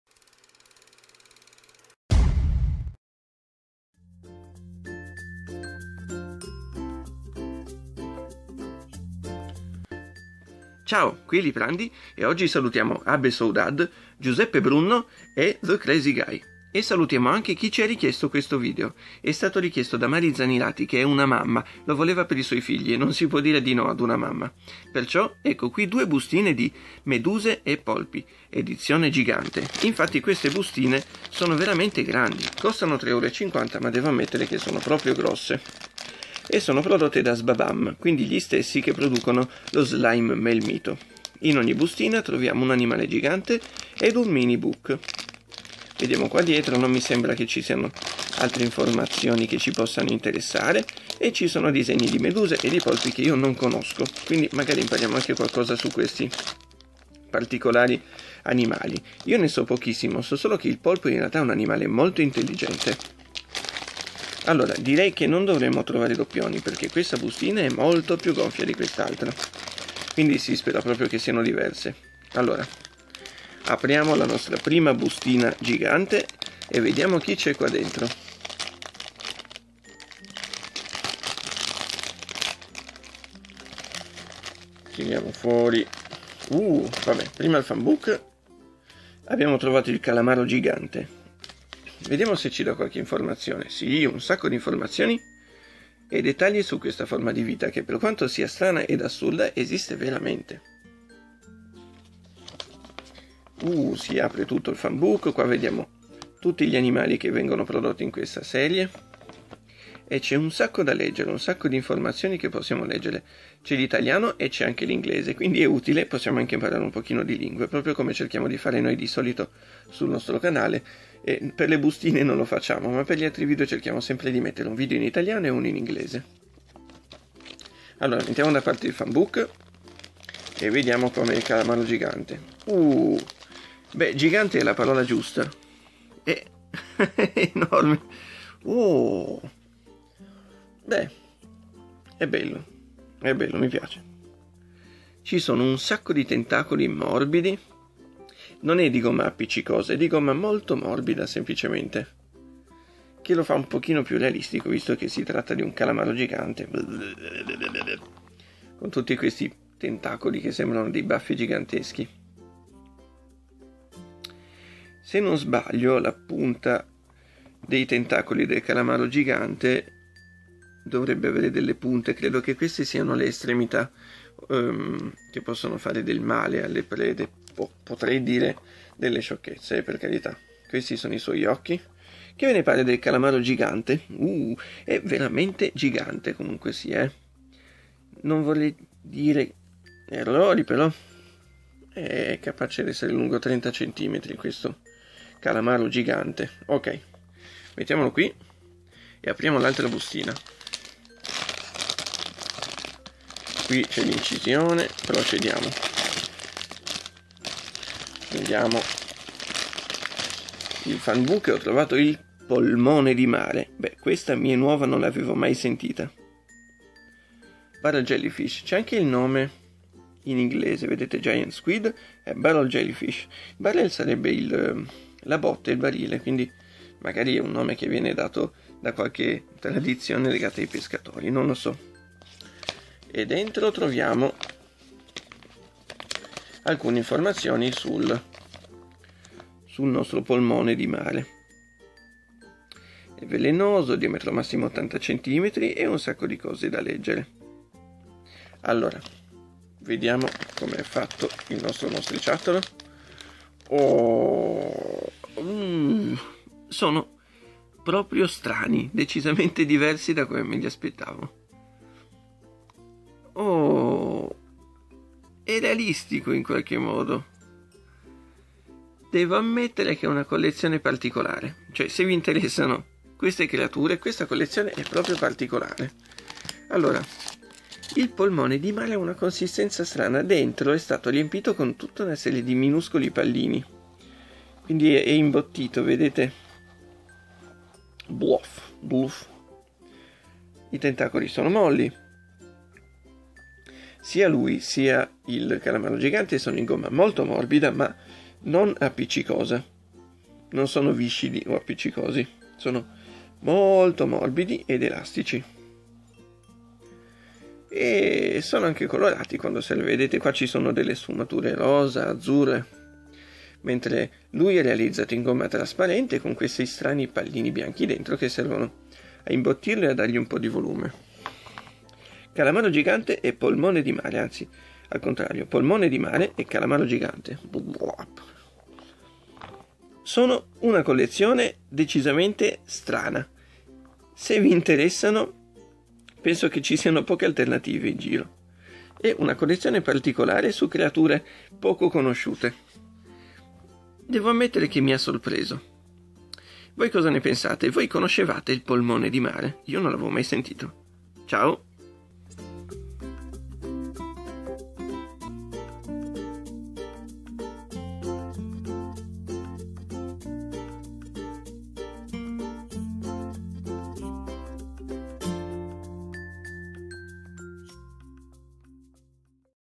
SBABAM - MEDUSE E POLPI UN SALTO IN EDICOLA RECENSIONE (ita)
Recensione in italiano di due bustine della serie Meduse e polpi della Sbabam!

As usual click on the red label in the upper left corner to switch to English language.
Al solito, potete cliccare sul rettangolino verde in alto a sinistra per passare alla lingua italiana.

ITA:
... ah i giocattoli!

ENG:
... oh, toys!

Ciao!